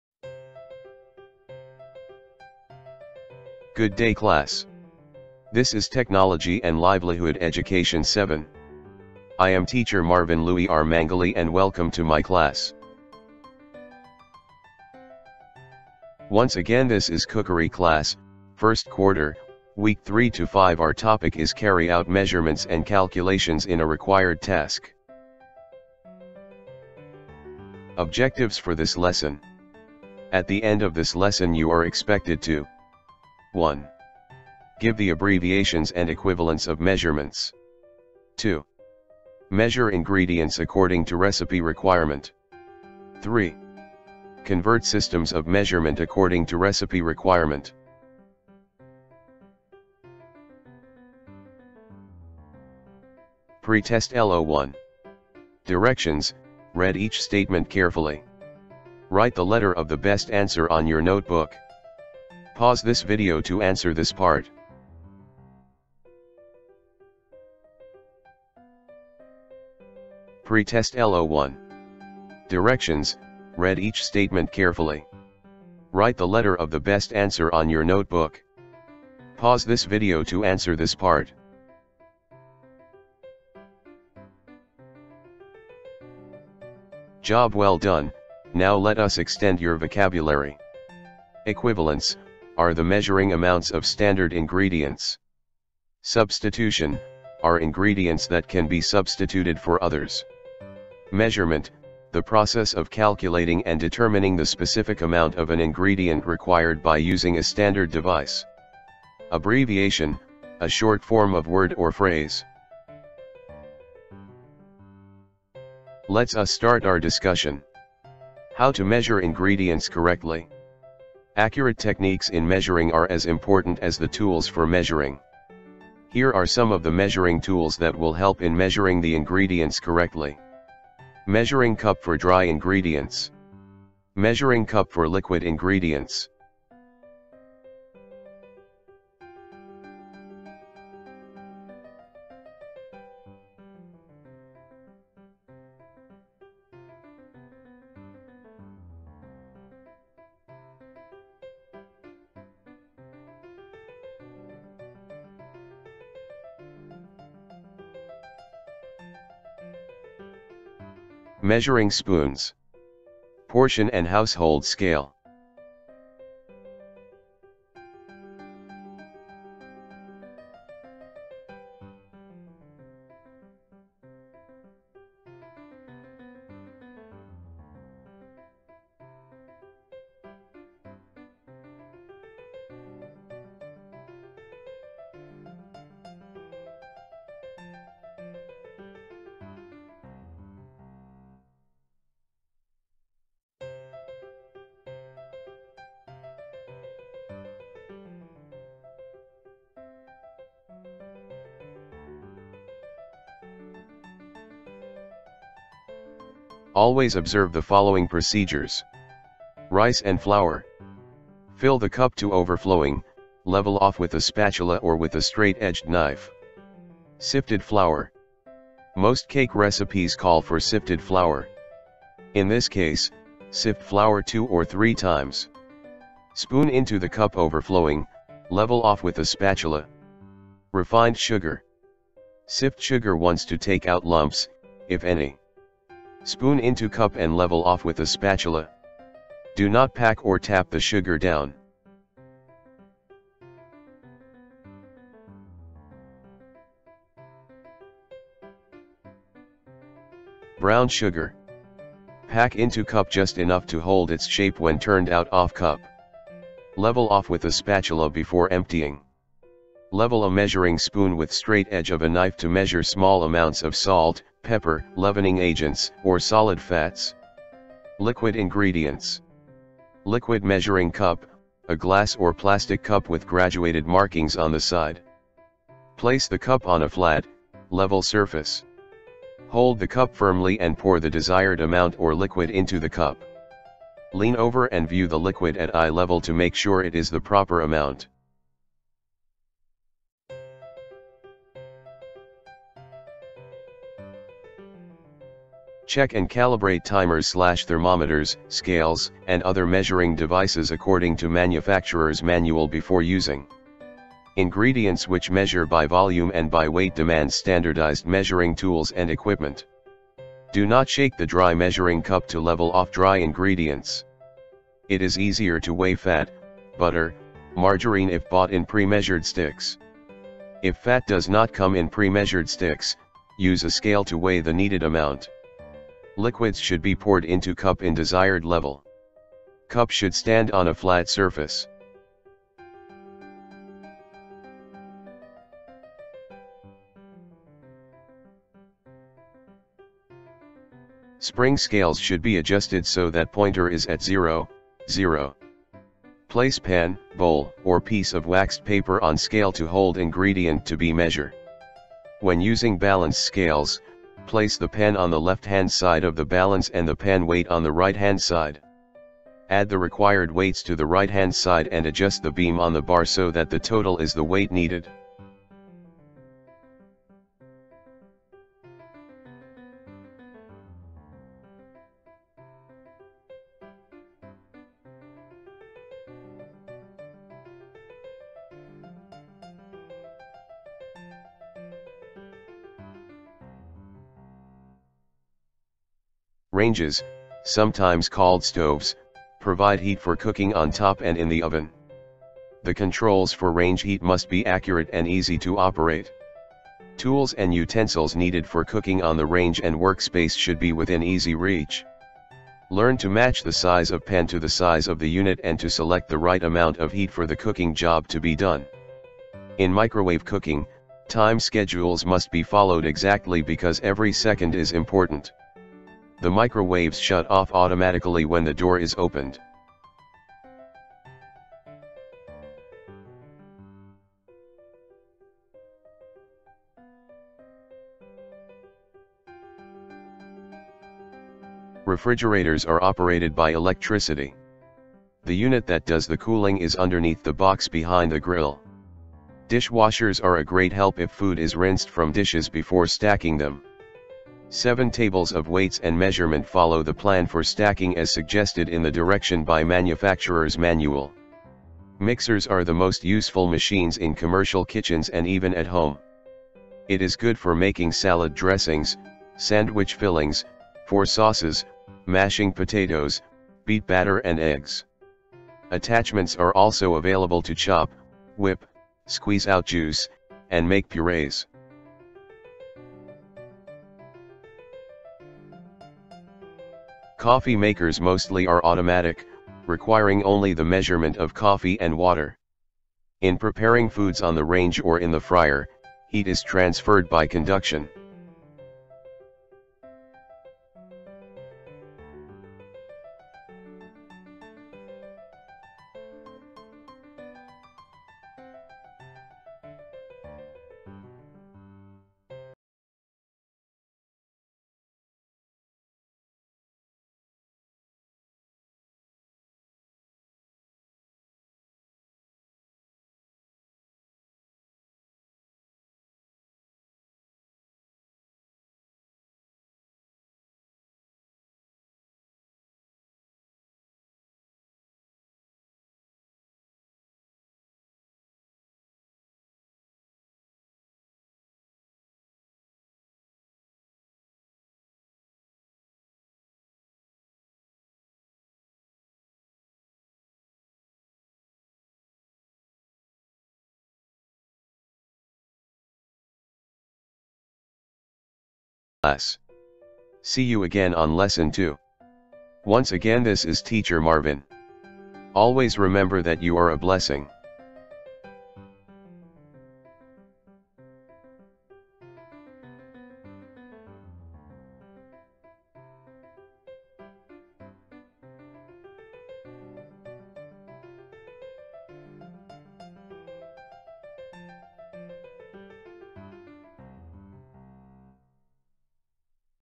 Cookery 7 - Carry Out Measurement and Calculations in a Required Task, Week 3-5 (Lesson1)
Cookery 7 - Carry Out Measurement and Calculations (Lesson 2 and 3)

Ref:
Learning Module
Curriculum Guide
Teacher’s Guide
Most Essentials Learning Competencies